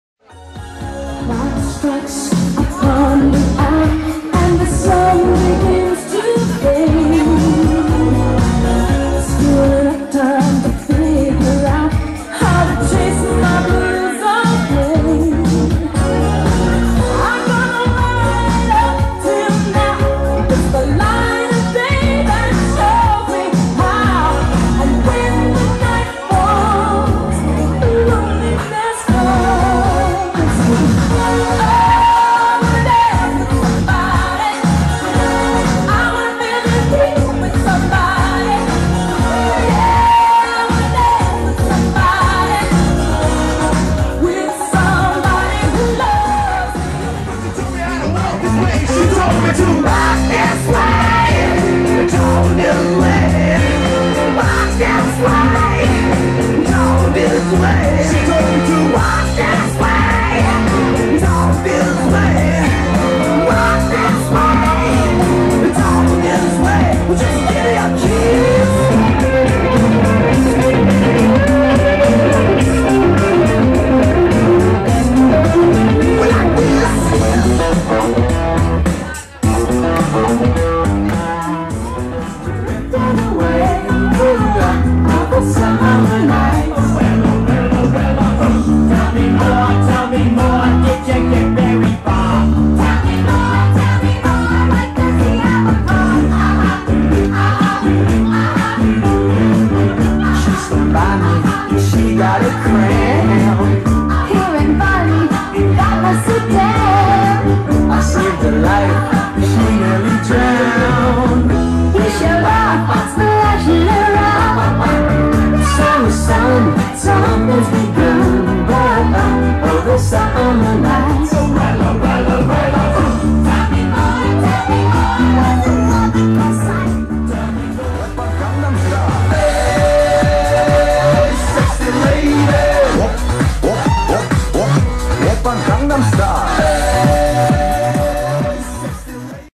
Fun On The Dancefloor at Vesna's Birthday Celebration, Sydney
Guests dancing and having fun at Vesna's birthday celebration held in Sydney.